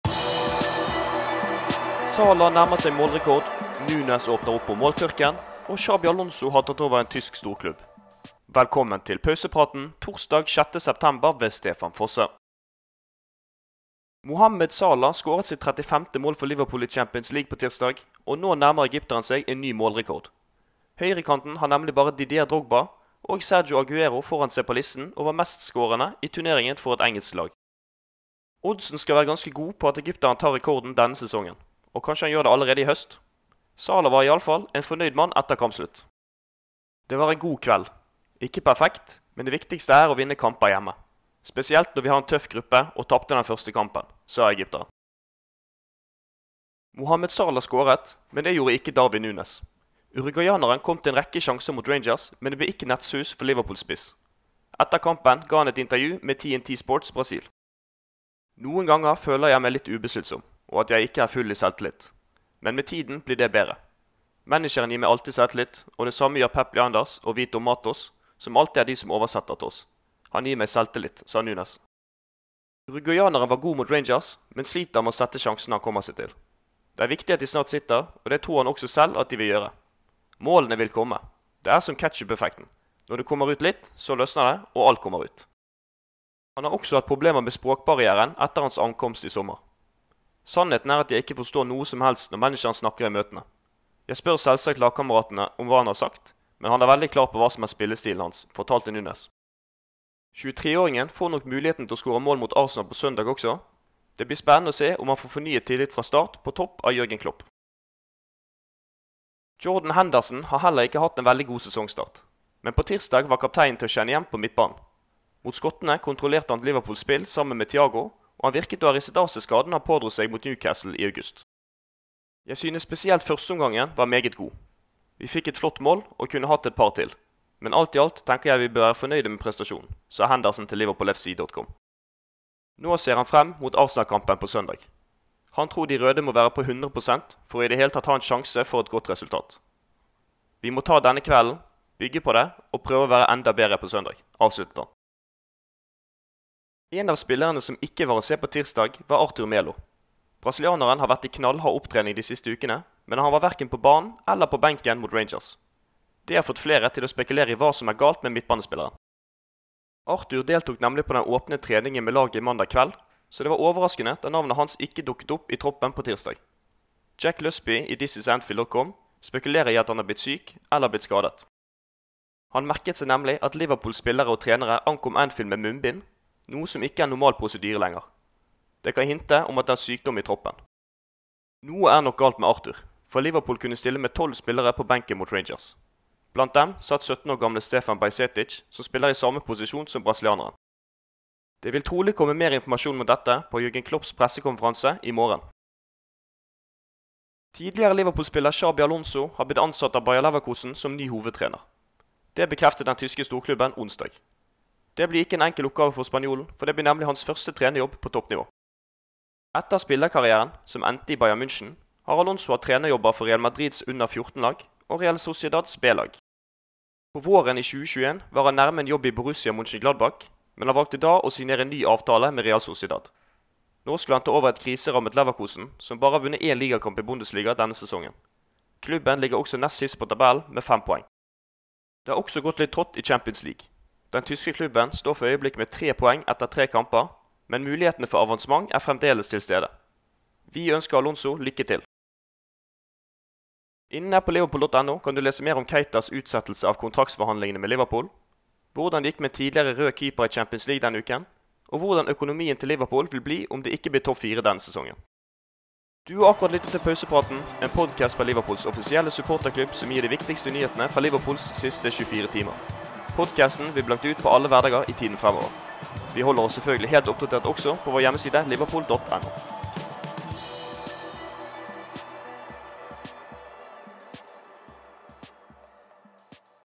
Reaksjonene etter Rangers-seieren og hva skjer med Arthur Melo?
Mohamed Salah nærmer seg enda en målrekord. Kan egypteren ta den allerede denne høsten? Darwin Nunez har åpnet opp om måltørken, og Xabi Alonso har tatt over tysk storklubb!